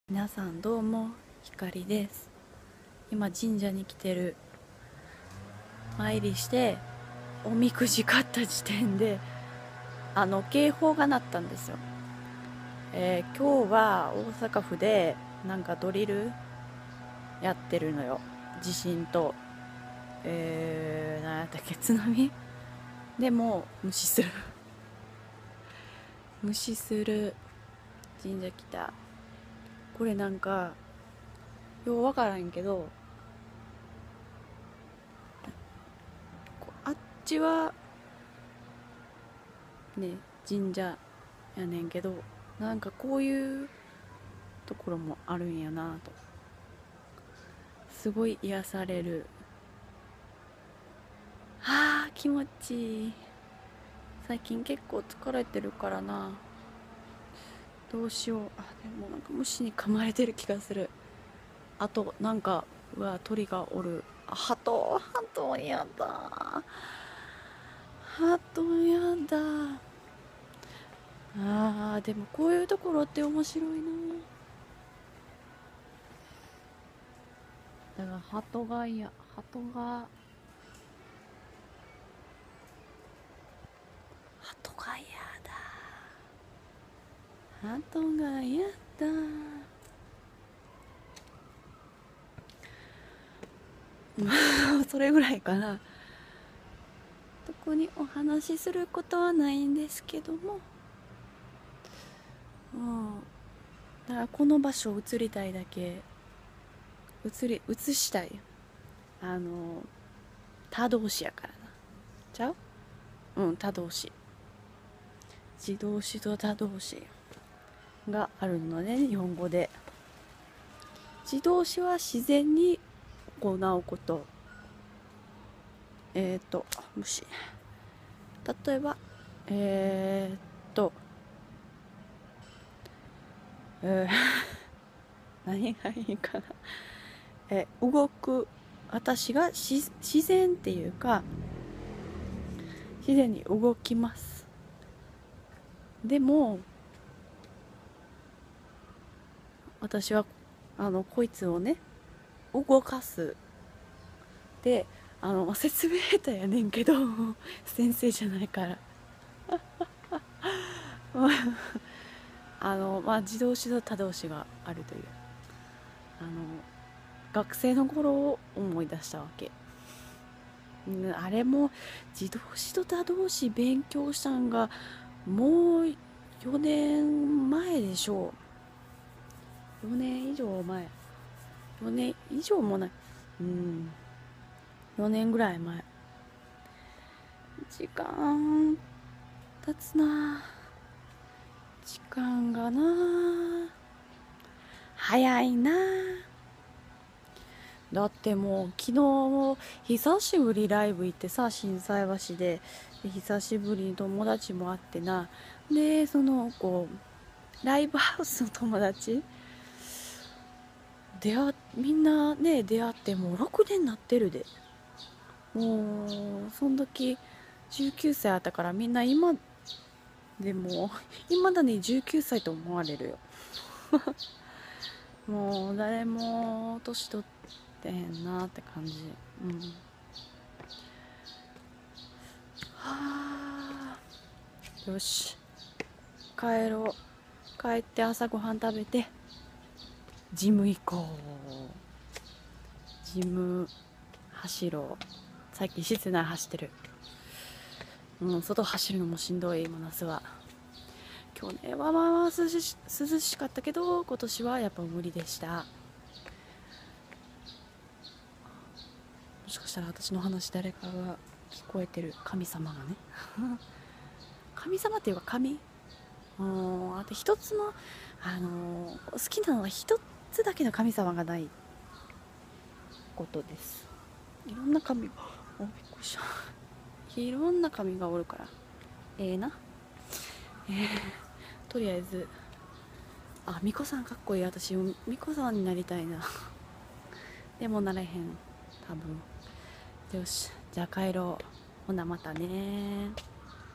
ドリルVSお参り
就活のときお世話になった神社です。
３ヶ月の間ほぼ毎日お参りしてた。

それ以来あまり来てないけど、やはり癒されるね。
ハトがいなければいいんですけども・・・